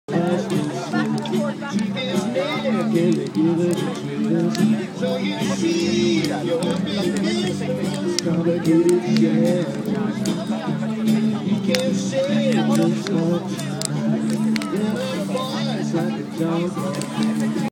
flatlakes bonfire june 2010 ireland
bonfire at the first night of flatlake festival june 2010